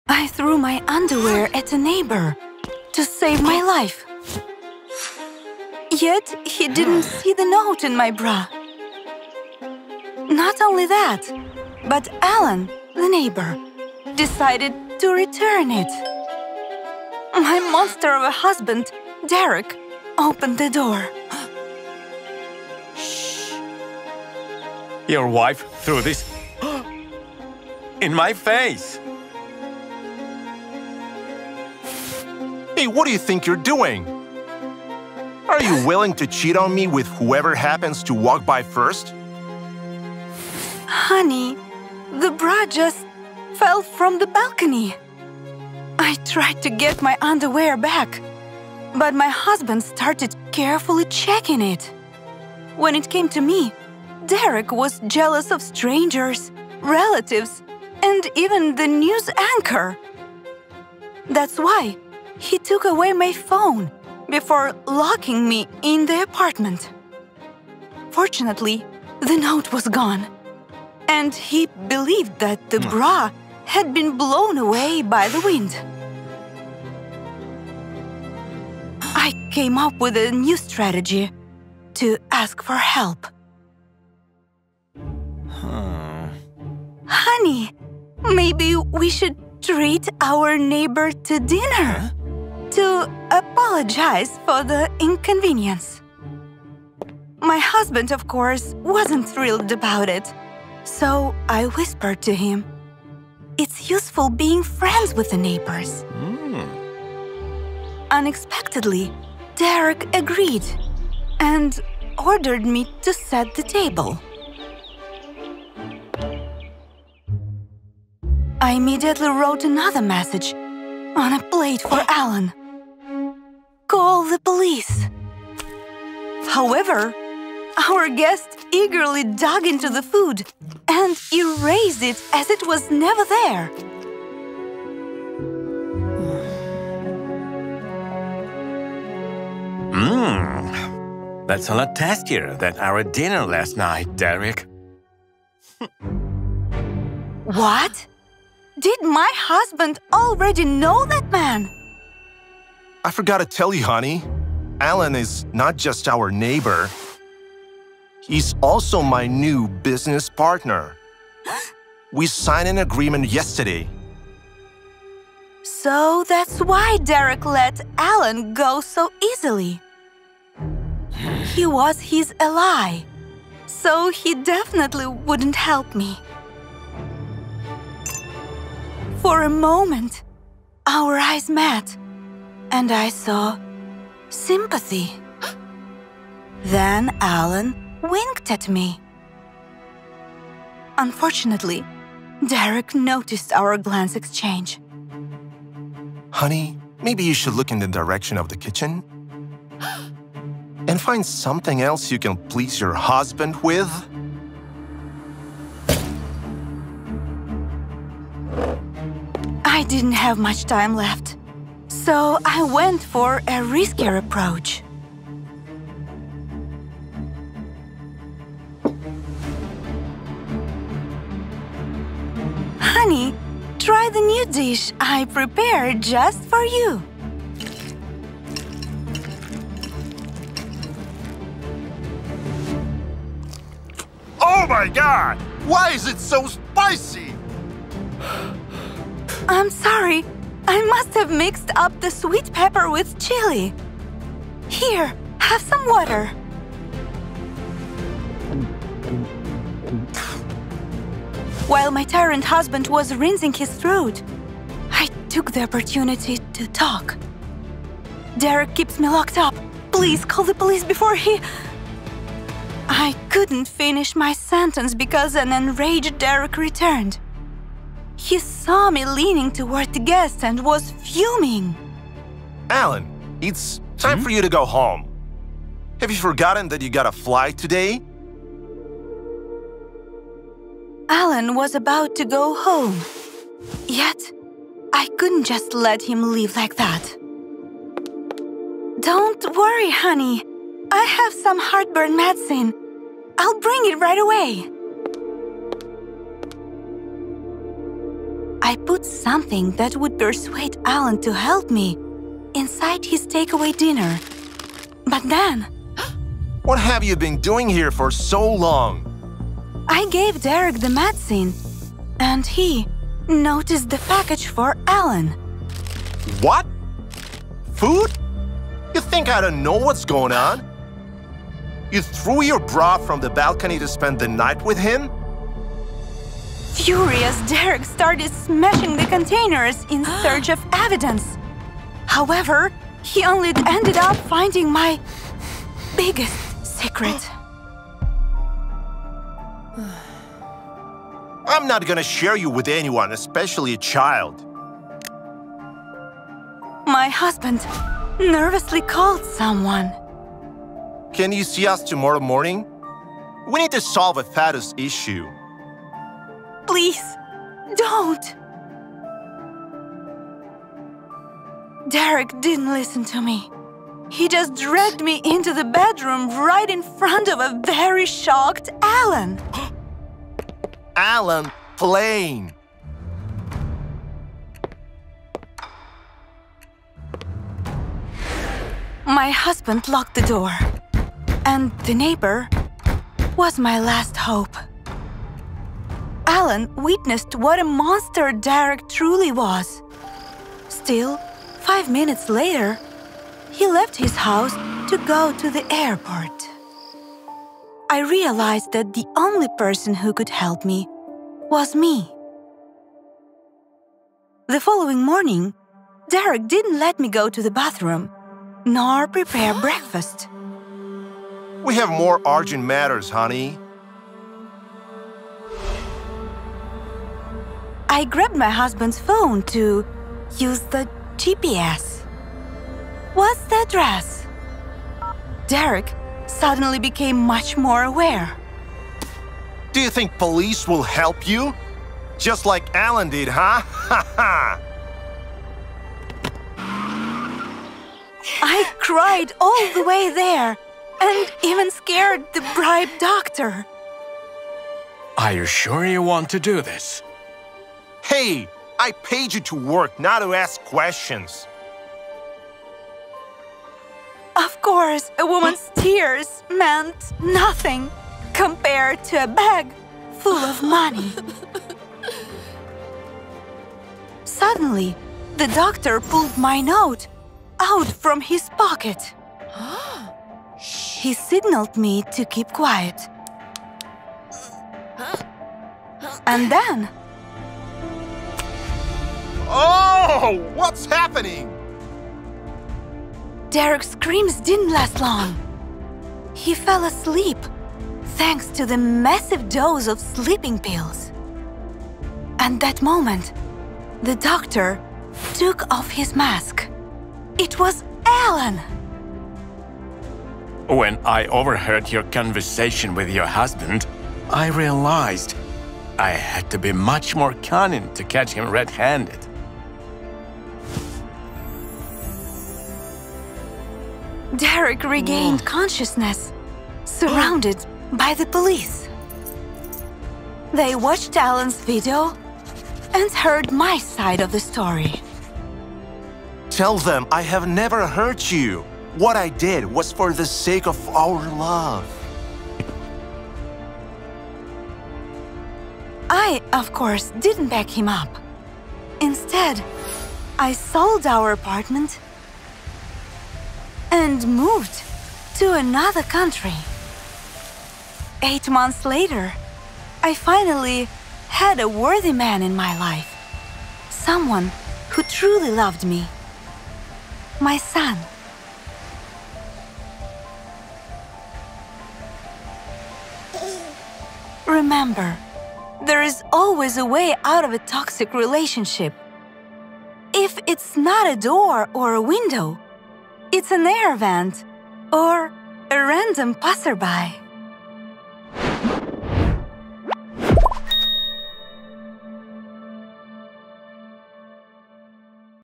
I Gave My Underwear To A Neighbor To Save My Life
Watch this emotional cartoon animation to see how a woman threw her bra at her downstairs neighbor with a  "help me" note in it.

The woman from this story was desperately seeking external assistance to escape her abusive marriage. She cleverly concealed a note in her bra and handed it to a neighbor. Unfortunately, he didn't see it and went to her house to return it. This infuriated her husband, prompting her to make a final effort to seek help. Her plan was to invite the neighbor over for dinner in hopes of befriending him. Surprisingly, her husband agreed. While serving him his meal, the woman discreetly left a message under the plate, urging him to contact the police.

That was her last glimmer of hope! Sadly, the man remained entirely focused on the food in front of him and failed to notice the plea for help...